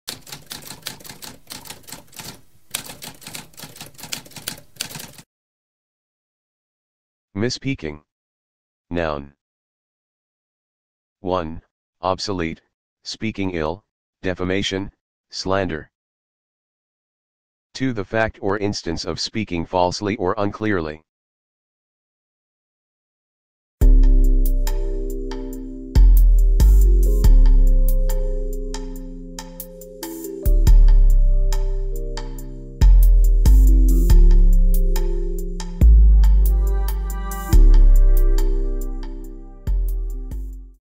What does misspeaking mean?
A spoken definition of misspeaking.

Intro Sound:
Typewriter - Tamskp